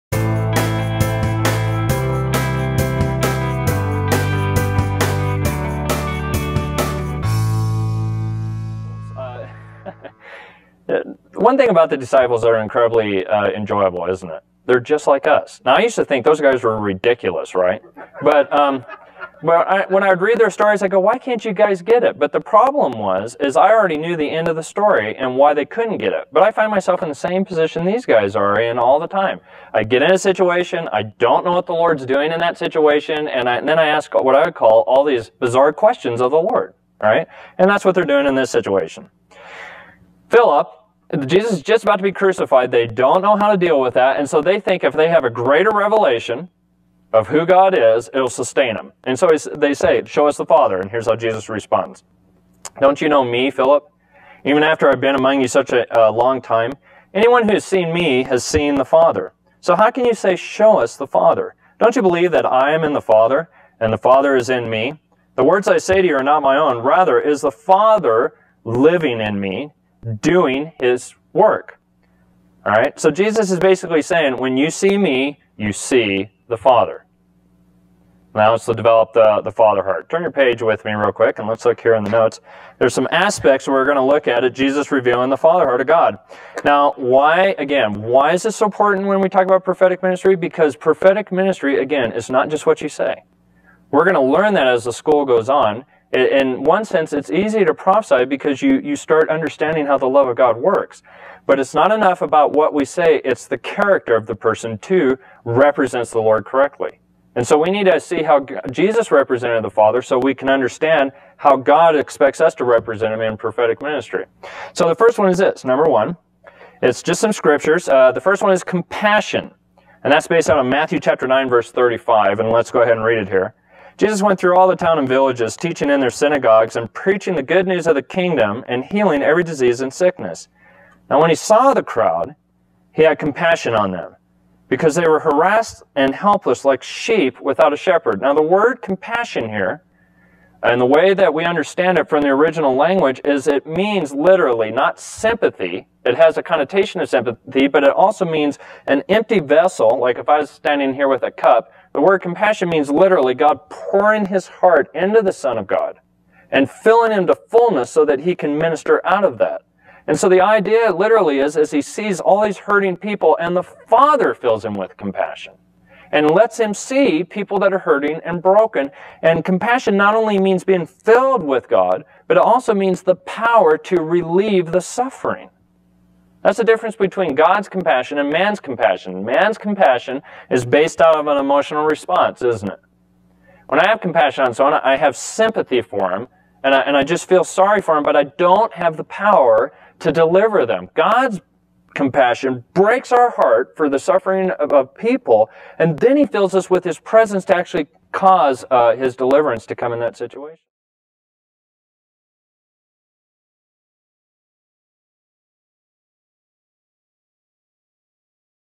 The Gold Standard for Leaders
We discover the power of the heart in the prophetic ministry. We talk about the need for compassion and strength in prophetic ministry.